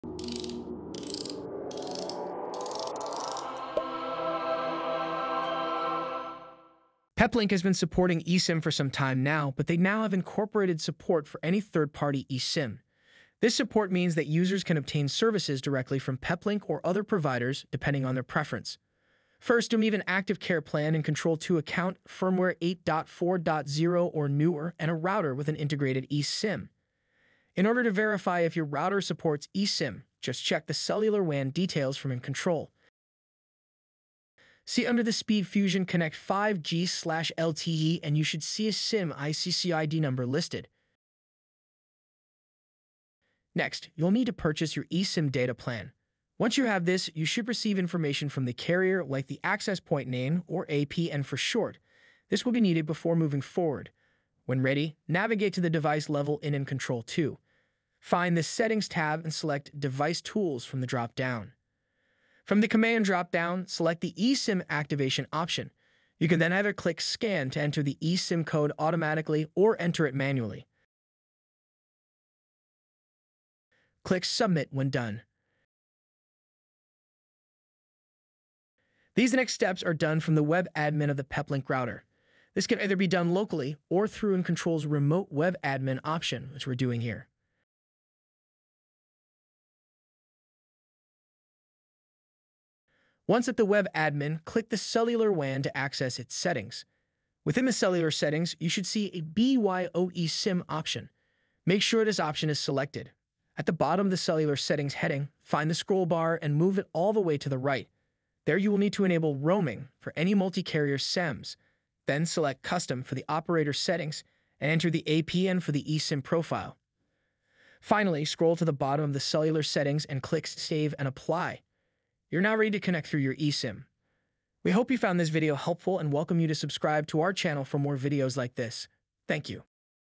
Peplink - How to Activate the Bring Your Own eSIM Option
This video demonstrates how to activate the eSIM for third party support on compatible Peplink and Pepwave devices.